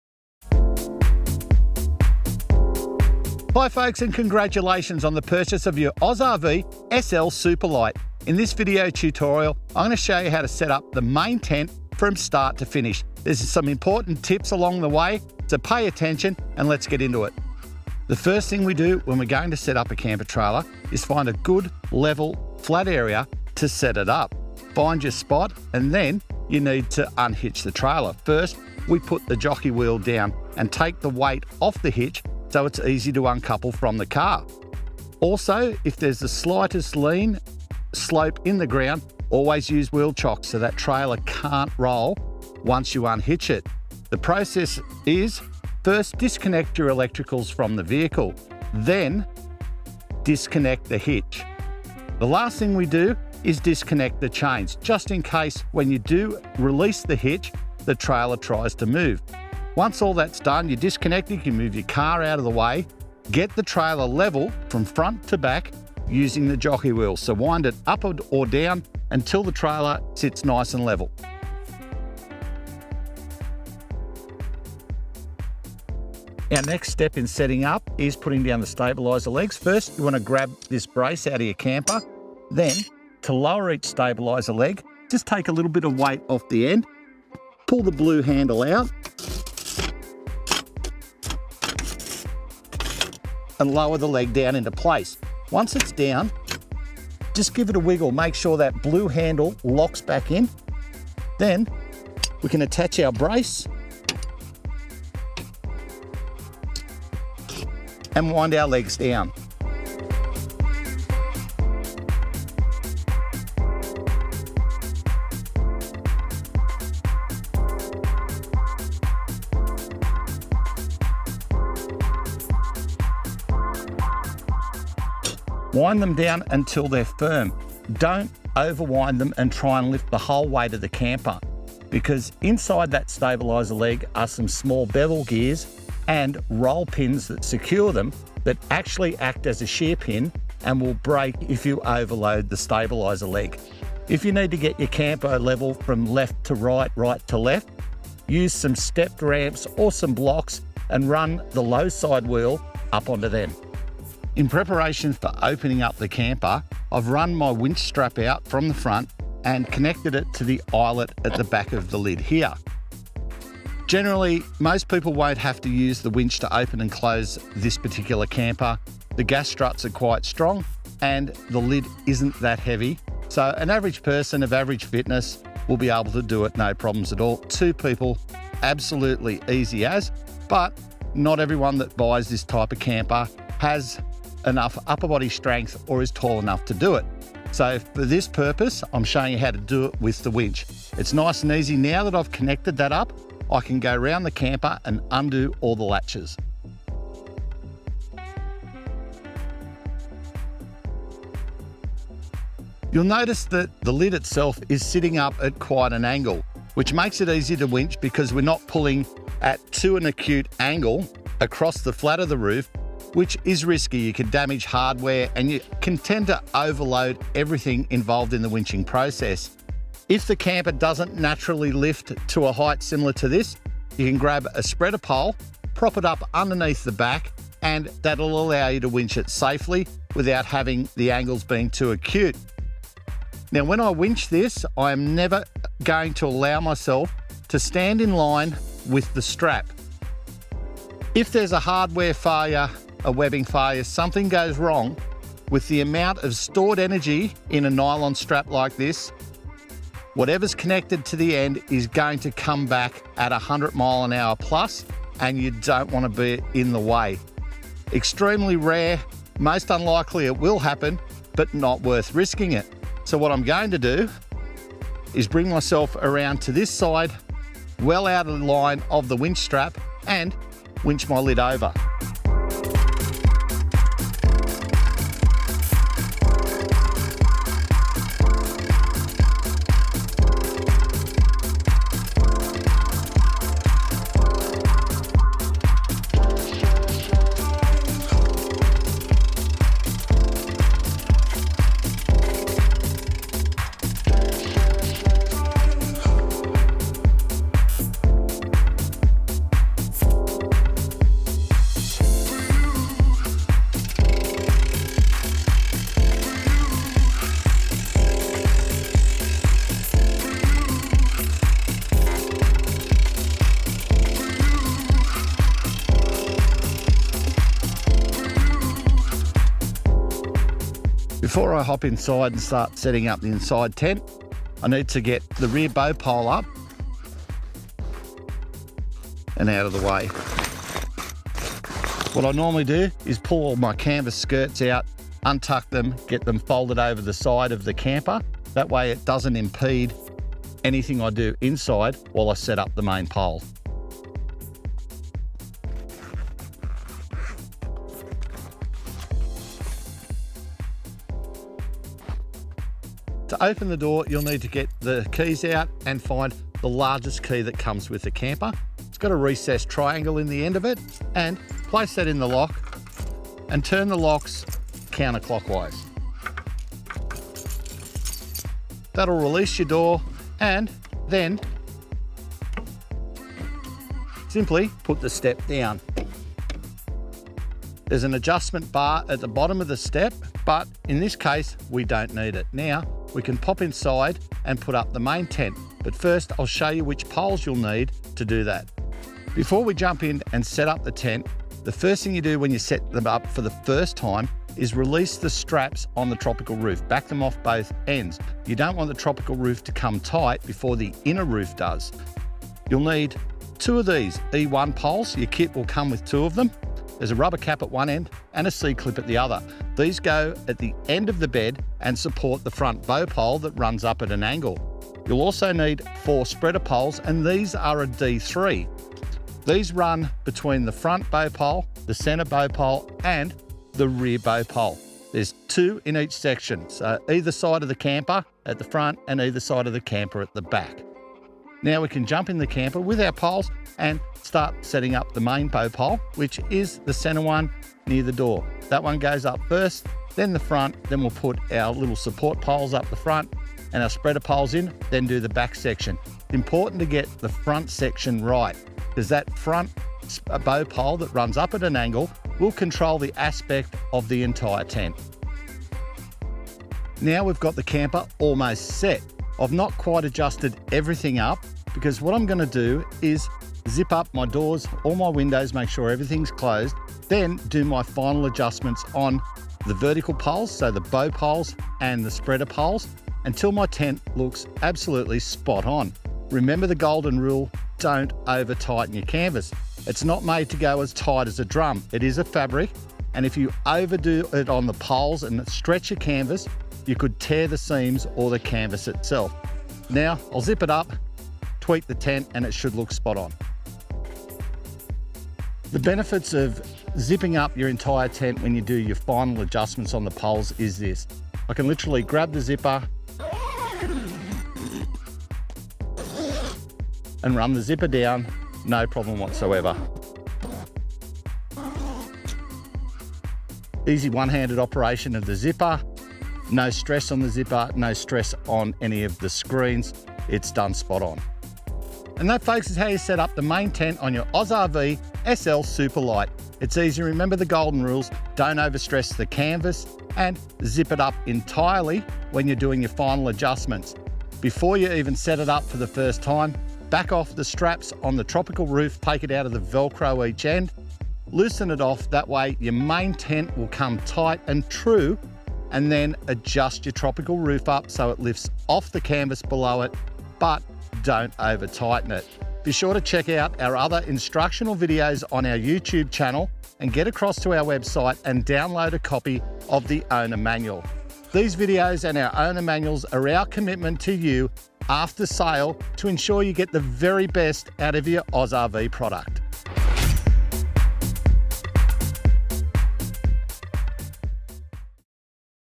How to: Set up AUSRV SUPERLITE Overland Pop-up Camper
At a smidge over two thousands pounds, the SL is perfect to tow behind small and medium 4X4’s and medium size SUV’s. Even your full size sedan can tow this versatile tent top camper to your favourite camp site. With full off road capability, good looks and standard features that will have you overlanding and camping off grid, the AusRV SL forward fold could be the start of new adventures and a lifetime of memories for you and your family.

 ✅ Perfect for 4
 ✅ 12” Electric drum brake
 ✅ Hot dipped galvanized steel chassis
 ✅ 4 Burner Stove
 ✅ Zippered with Mosquito netting
 ✅ 19" TV